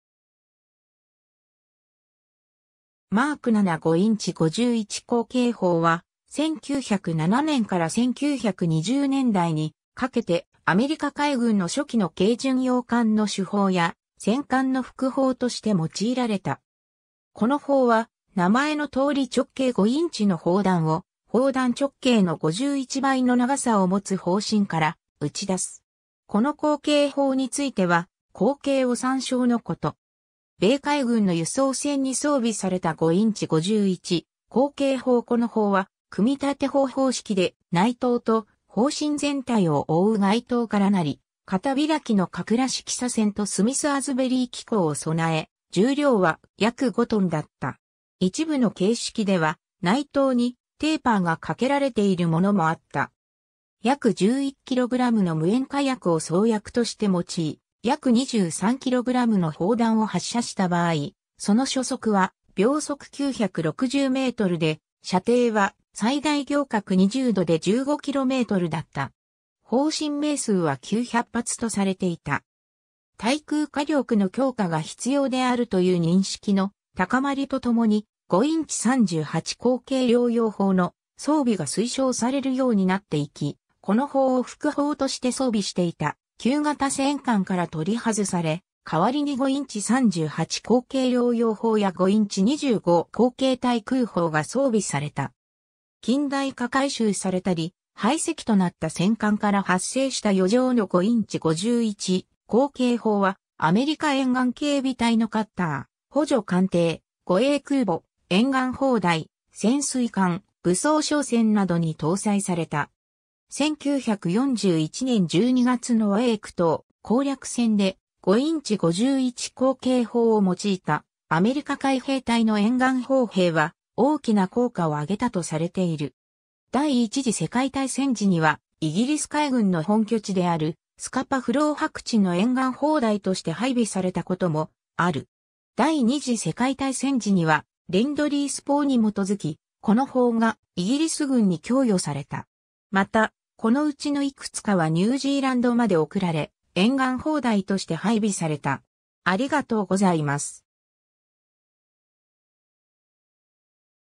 Mk 7 5インチ砲
Mk 7 5インチ51口径砲（Mk 7 5"/51 caliber guns）は、1907年から1920年代にかけて、アメリカ海軍の初期の軽巡洋艦の主砲や、戦艦の副砲として用いられた。
この砲は、名前の通り直径5インチの砲弾を、砲弾直径の51倍の長さを持つ砲身から撃ち出す。
この口径法については、口径を参照のこと。
 米海軍の輸送船に装備された 5インチ51口径砲（1942年中頃撮影） この砲は組立砲（en:built-up gun）方式で、内筒と砲身全体を覆う外筒から成り、片開きの隔螺式鎖栓とスミス・アズベリー機構（Smith-Asbury mechanism）を備え、重量は約5トンだった。
一部の形式では、内筒にテーパーがかけられているものもあった。
約11kgの無煙火薬を装薬として用い、約23kgの砲弾を発射した場合、その初速は960m/sで、射程は最大仰角20度で15kmだった。
砲身命数は900発とされていた。
 対空火力の強化が必要であるという認識（特に、真珠湾奇襲以降）の高まりと共に、5インチ38口径両用砲の装備が推奨されるようになっていき、この砲を副砲として装備していた旧型戦艦から取り外され、代わりに5インチ38口径両用砲や5インチ25口径対空砲が装備された。
近代化改修されたり、廃籍となった戦艦から発生した余剰の5インチ51口径砲は、アメリカ沿岸警備隊のカッター、補助艦艇、護衛空母、沿岸砲台、潜水艦、武装商船などに搭載された。
1941年12月のウェーク島攻略戦で、5インチ51口径砲を用いたアメリカ海兵隊の沿岸砲兵は大きな効果を挙げたとされている。
 第一次世界大戦時には、イギリス海軍の本拠地であるスカパ・フロー泊地の沿岸砲台として配備されたこともある。
 第二次世界大戦時には、レンドリース法に基づき、この砲がイギリス軍に供与された。
また、この内のいくつかはニュージーランドまで送られ、沿岸砲台として配備された。